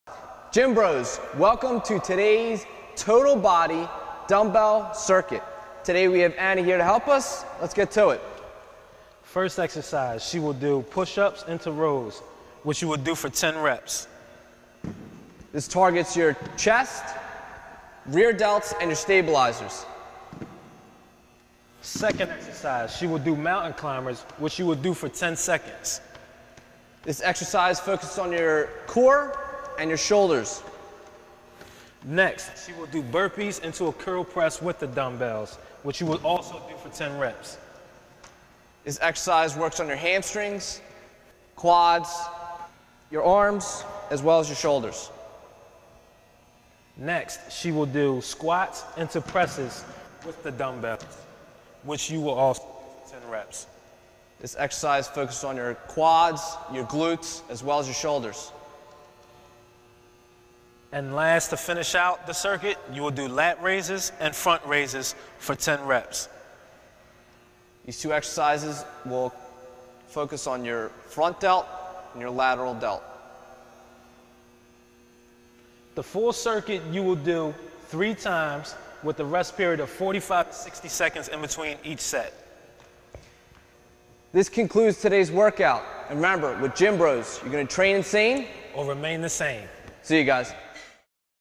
Total Body Dumbbell Circuit
This circuit workout is for people who need an intense workout in a short amount of time.